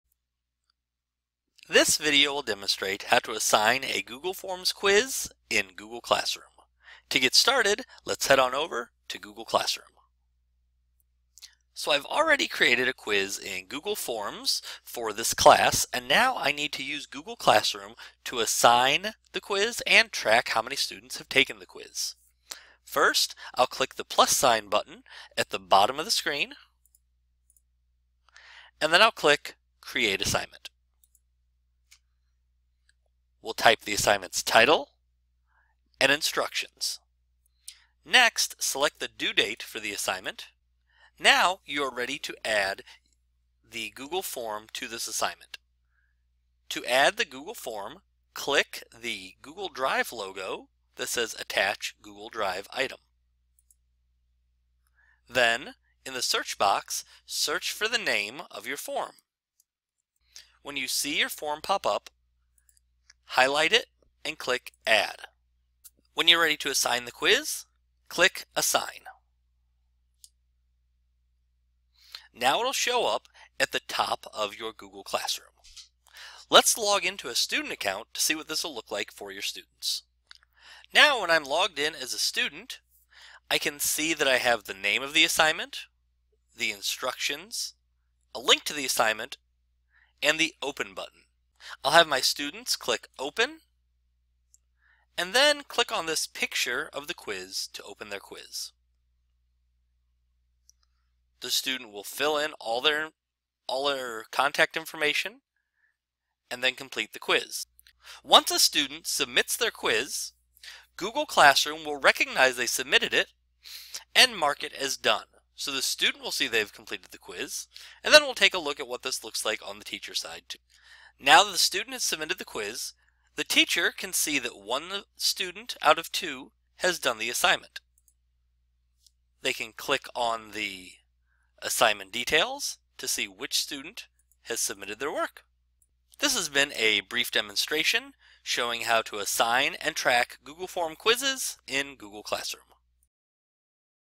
Assign and Track Google Forms Quiz in Google Classroom
This video demonstrates how to assign and track a Google Forms quiz in Google Classroom.